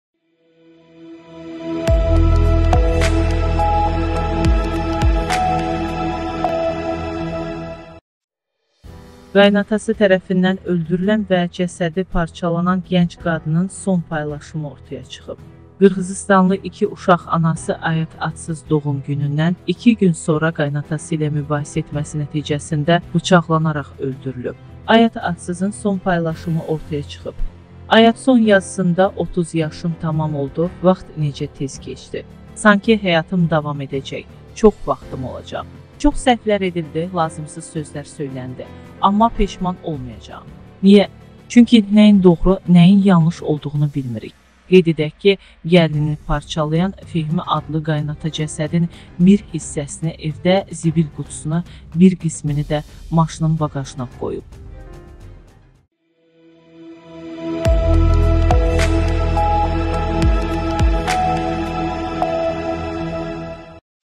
Qayınatası tərəfindən öldürülən gəlinin SON PAYLAŞIMI: ÜRƏKLƏRİ DAĞLADI
Qayınatası tərəfindən öldürülən və cəsədi parçalanan gənc qadının son paylaşımı ortaya çıxıb.

Qırğızıstanlı 2 uşaq anası Ayat Adsız doğum günündən 2 gün sonra qayınatası ilə mübahisə etməsi nəticəsində bıçaqlanaraq öldürülüb. Ayat Adsızın son paylaşımı ortaya çıxıb.

Ayat son yazısında "30 yaşım tamam oldu, vaxt necə tez keçdi. Sanki həyatım davam edəcək, çox vaxtım olacaq. Çox səhvlər edildi, lazımsız sözlər söyləndi, amma peşman olmayacağam. Niyə? Çünki nəyin doğru, nəyin yanlış olduğunu bilmirik”.

Qeyd edək ki, gəlinini parçalayan Fehmi A cəsədin bir hissəsini evdə zibil qutusuna, bir qismini də maşının baqajına qoyub.